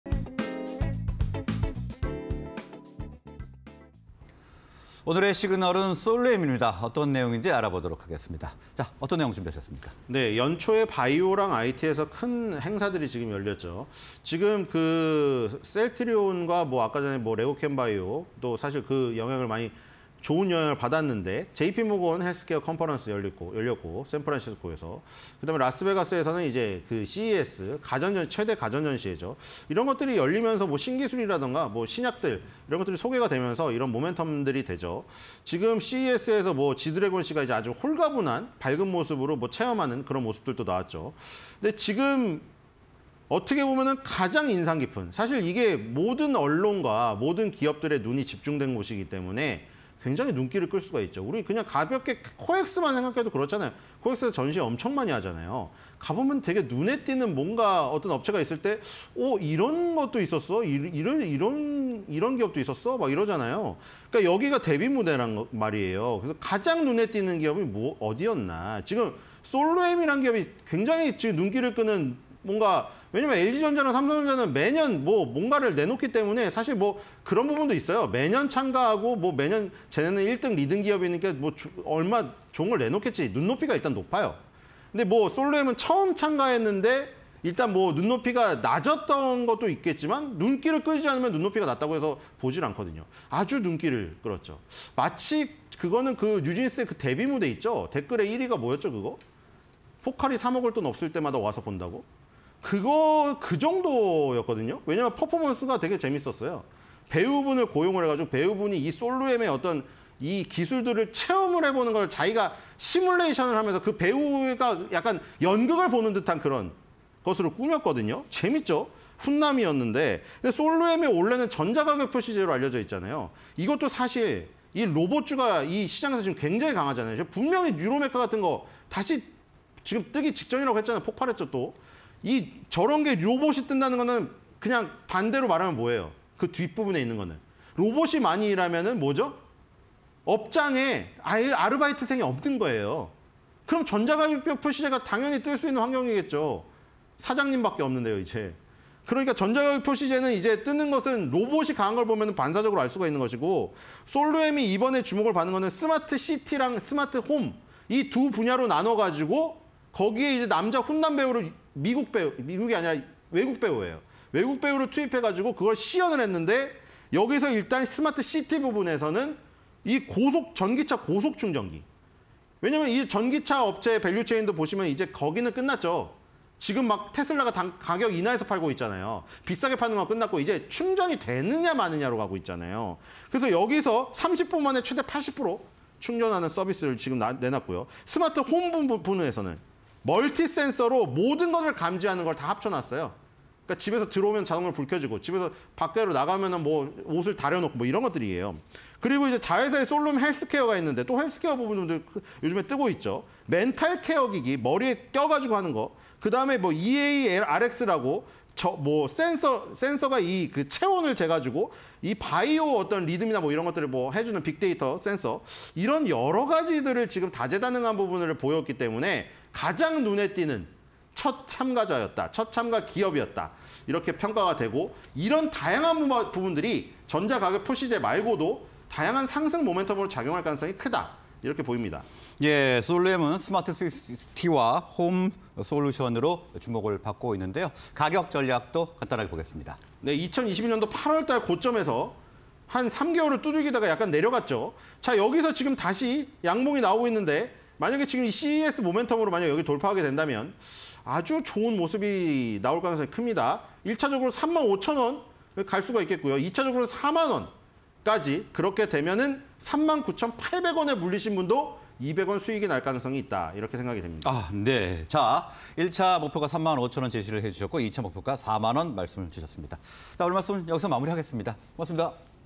솔루엠(248070), CES서 호평... "다재다능한 솔루션"_신소장의 시그널 (20240115)
솔루엠(248070) "스마트한 기술력 뽐내다"
- 성공적인 CES 데뷔전
- 스마트 시티&홈 솔루션으로 눈도장
- 60kW 전기차 고속 충전기 공개